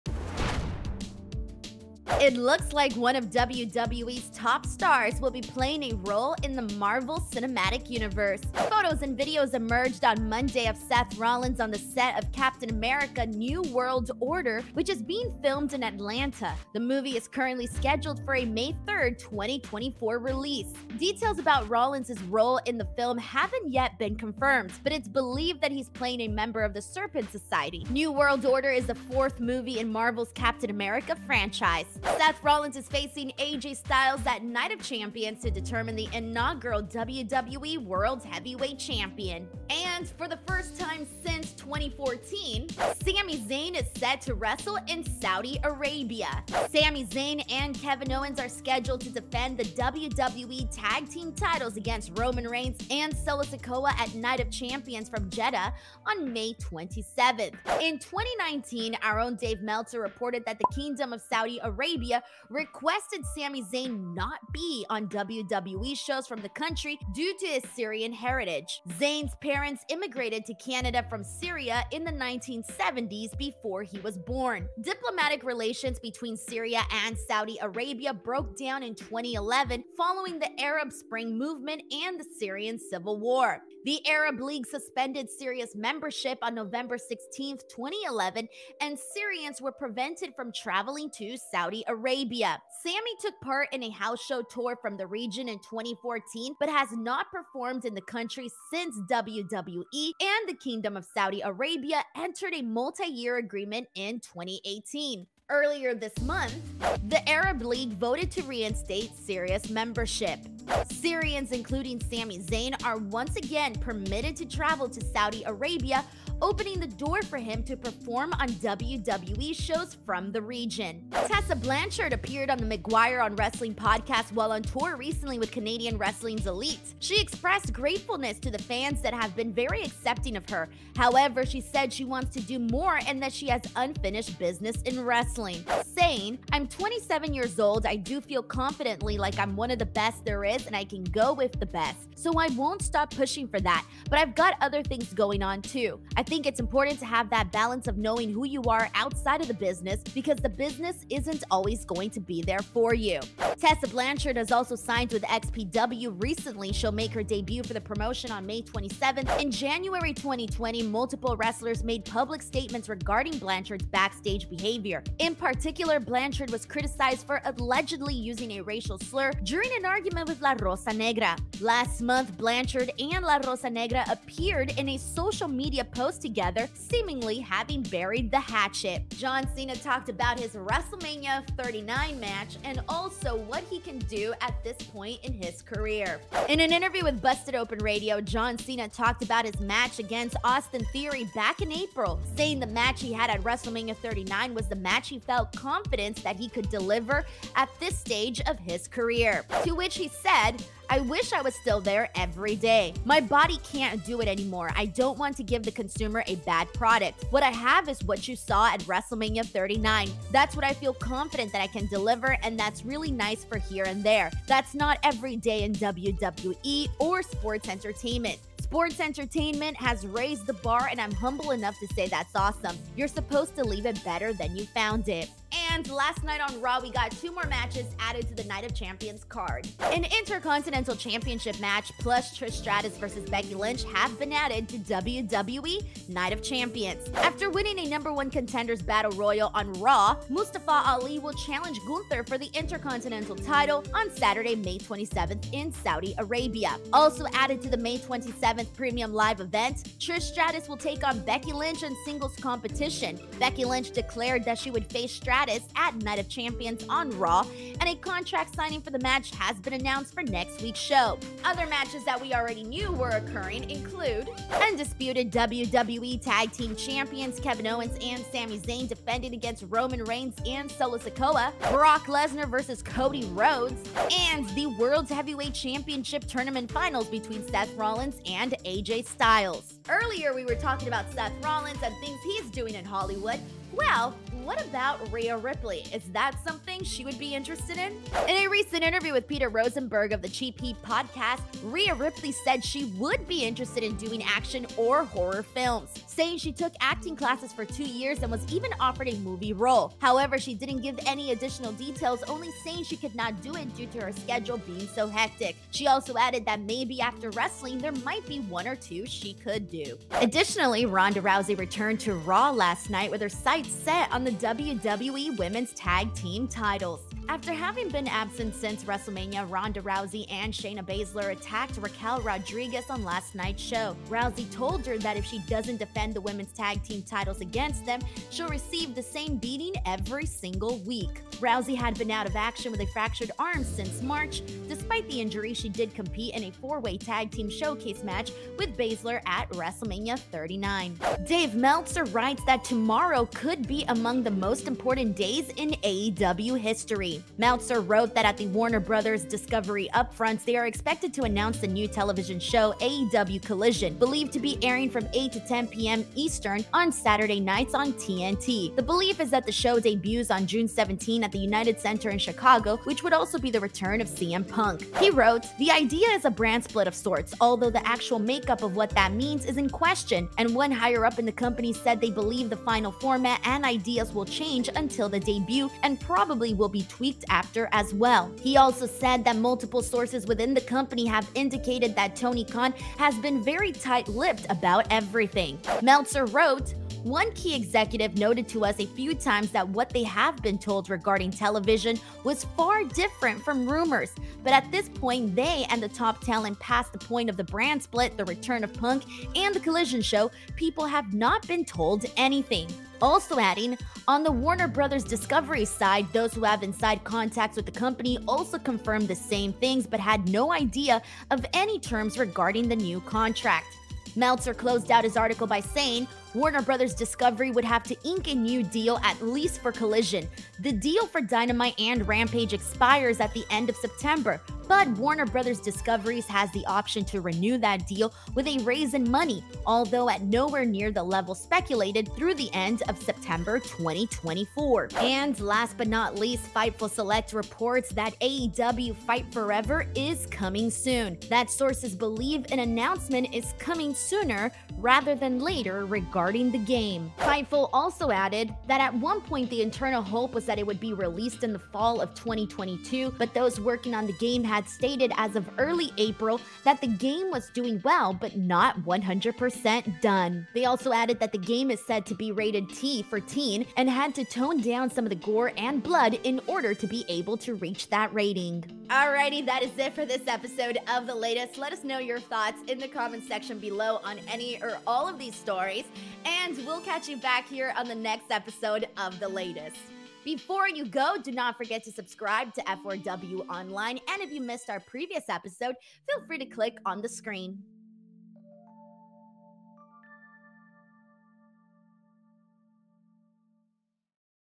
Seth Rollins Appearing in Marvel, Explaining Why Sami Zayn Can Now Go To Saudi Arabia & Lots More!
Welcome to another episode of The Latest with @DeniseSalcedo breaking down the biggest wrestling news headlines of the day.

Topics:
-- Seth Rollins will be playing a role in the Marvel Cinematic Universe.
-- For the first time since 2014, Sami Zayn is set to wrestle in Saudi Arabia.
-- Tessa Blanchard updates
-- John Cena talked about his match at WrestleMania 39 and what he can still do at this point in his career.
-- WWE Night of Champions matches
-- Rhea Ripley talks movies
-- Ronda Rousey returned to Raw last night with her sights set on the WWE Women's Tag Team titles.
--Dave Meltzer writes that tomorrow could be among the most important days in AEW history.
-- AEW Fight Forever video game update